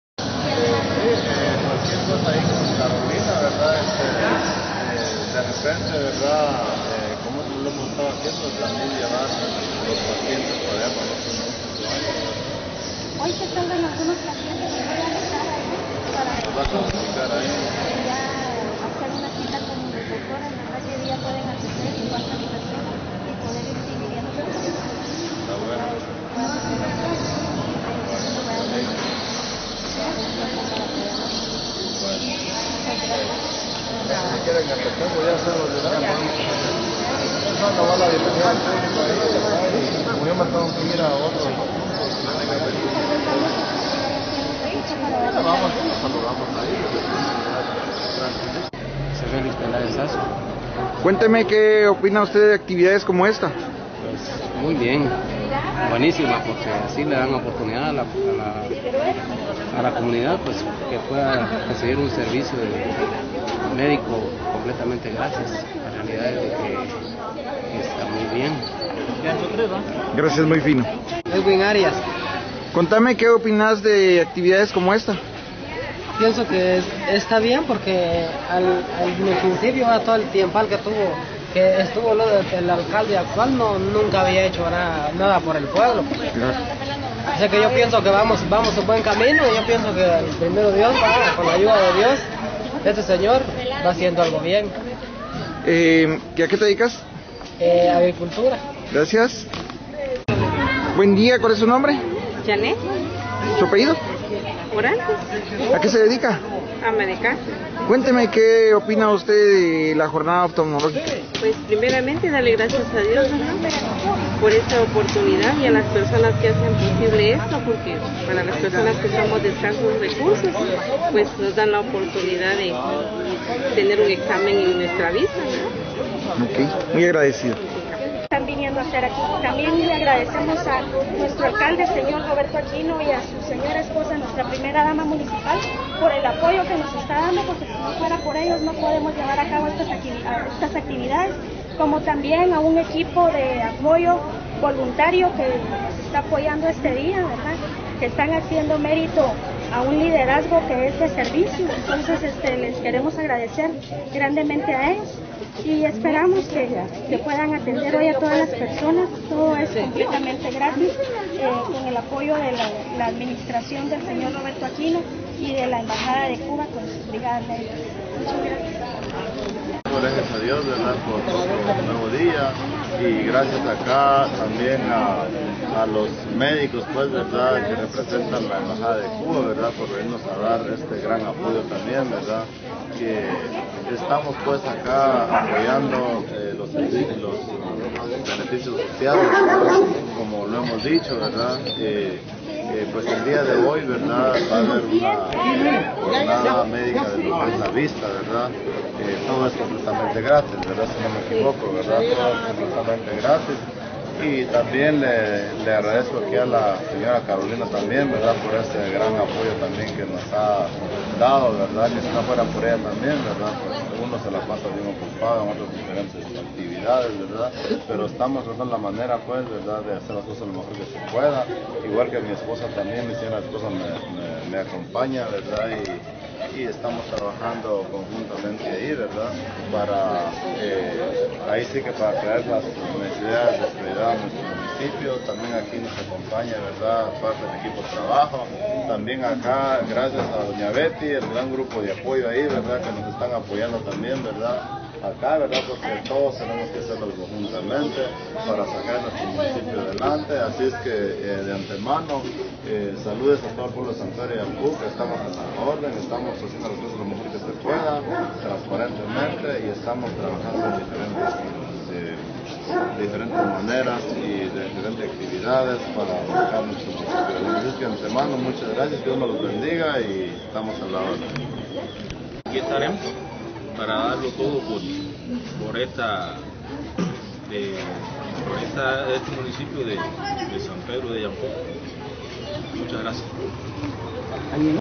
Jornada oftalmologica
jornada de medicos cubanos que vinieron a atender a los sanpedranos de enfermedades visuales...